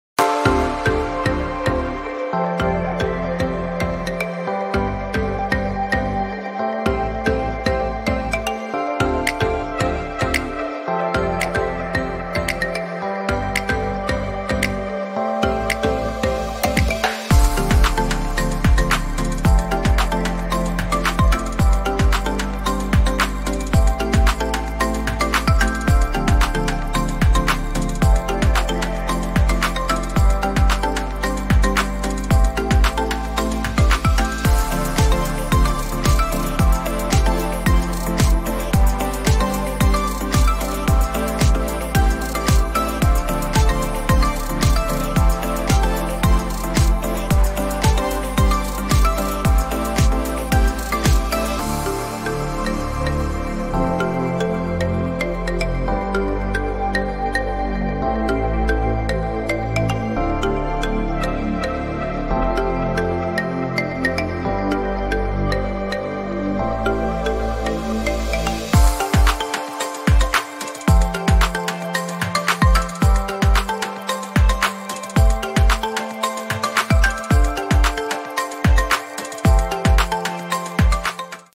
Une robe en chocolat à croquer ! 🍫
Des robes en chocolats s'invitent au salon du chocolat à Paris. Regardez ces créations délicieuses ! Une robe à manger, ça vous dit ? 🍫